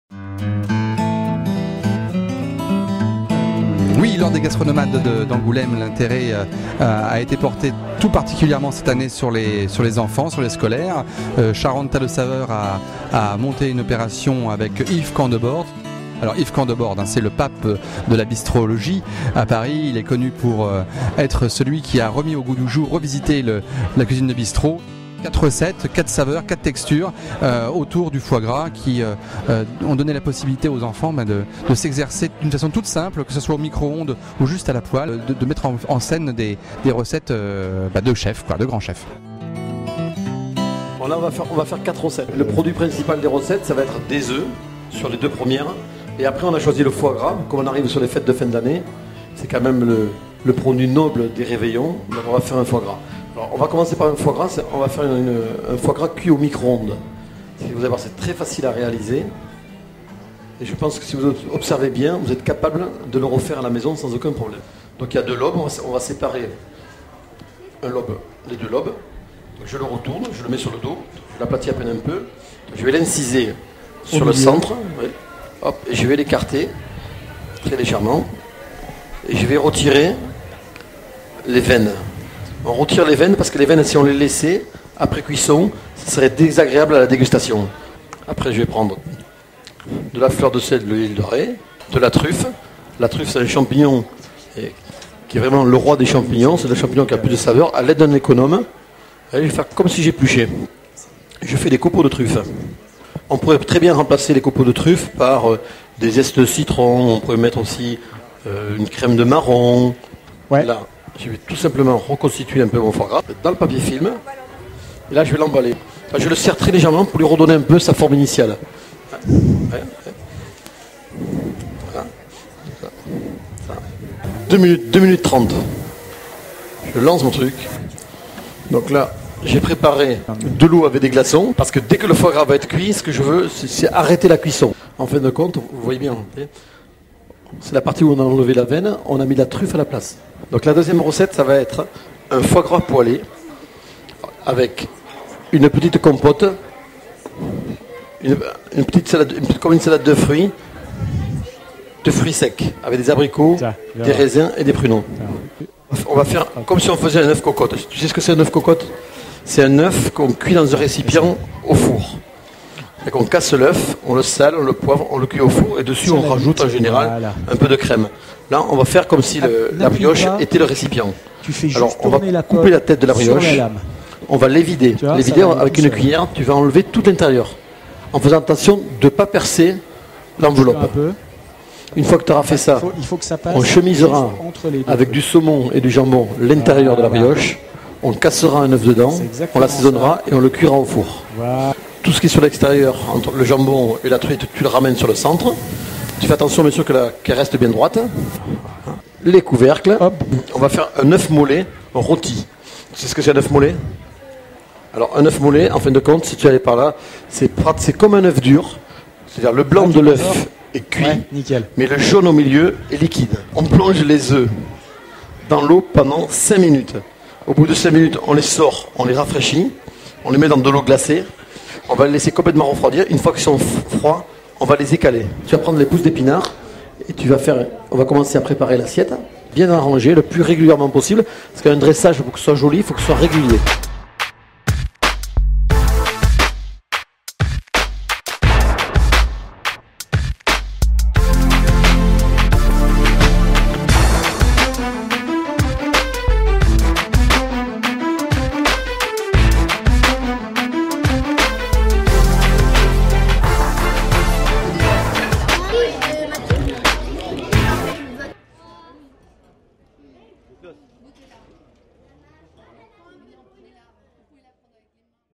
Camdeborde avec les enfants sous le chapiteau du Club 16 des Saveurs charentaises Gastronomades 2011
Yves Camdeborde fait cuisiner les enfants de la classe CM2 de l'Ecole Victor Duruy d'Angoulême. Ces enfants sont à l'écoute du chef car ils réalisent un film court métrage sur l'alimentation avec l'aide du CDDP (centre départemental de documentation pédagogique) et de Charente Terre de Saveurs